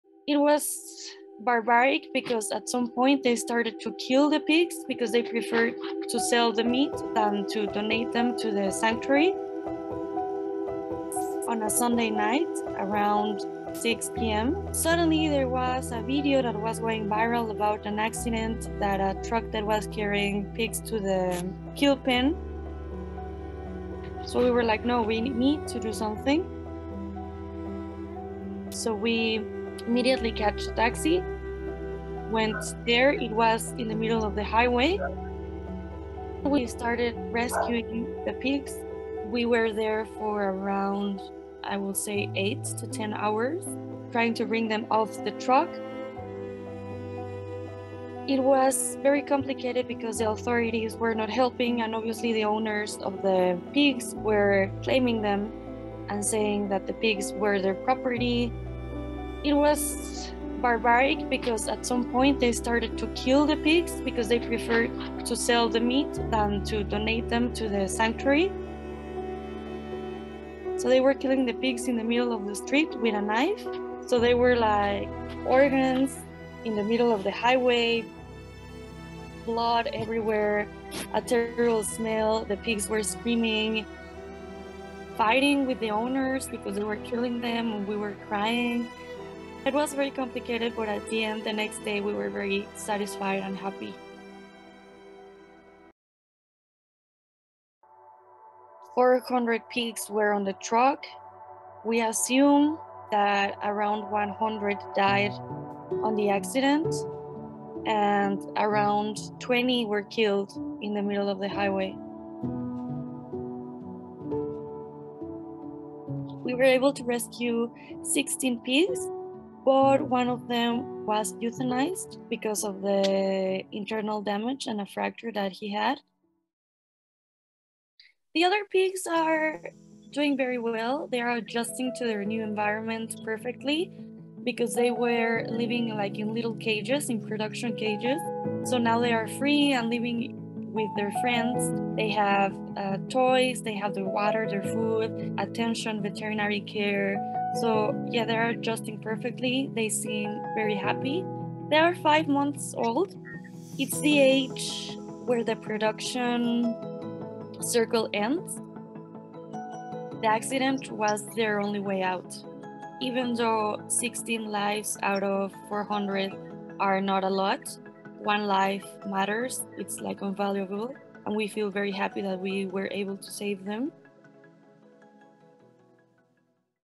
15 Pigs Saved From a Truck Crash in Mexico
Recently the Mexican vegan activist community was faced with this heartbreaking scene 💔

All these individuals endured additional pain, on top of a life of exploitation and being headed to their deaths, they were in a traffic accident that killed many of them 😔

The gruesome details of how it unfolded is what Seres Libres is sharing with us in this video 😢

We’re to thankful to their amazing team for welcoming 16 survivors to their sanctuary, where volunteers have enabled a space for them to be comfortable in while they find their forever homes. 🥺

Please show them your support in whichever way you can 👉 Seres Libres - Animal Rescue